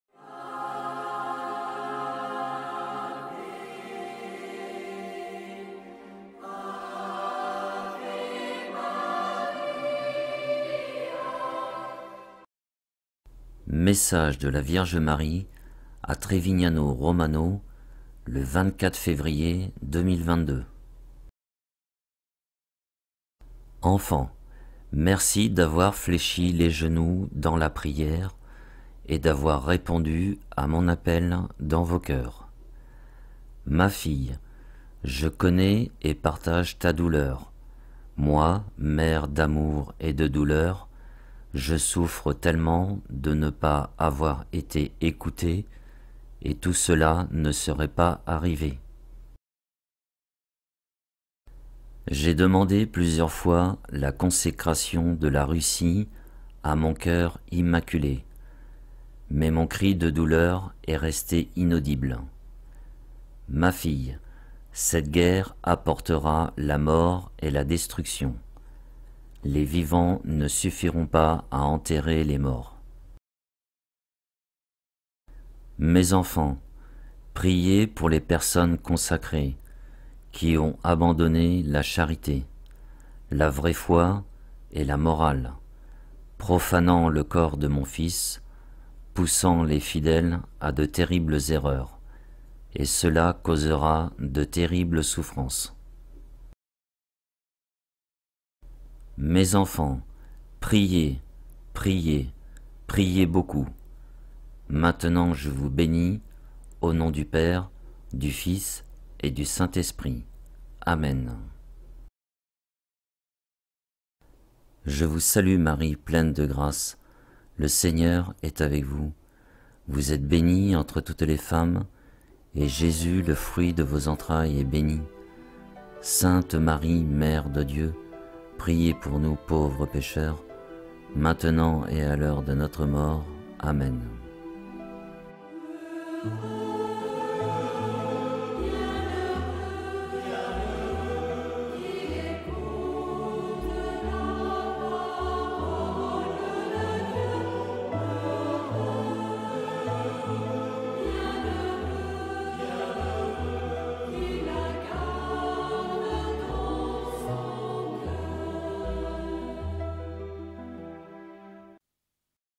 Trevignano Romano Message du 24 Février 2022
Message de la Vierge Marie à Trevignano Romano en Italie le 24 Février 2022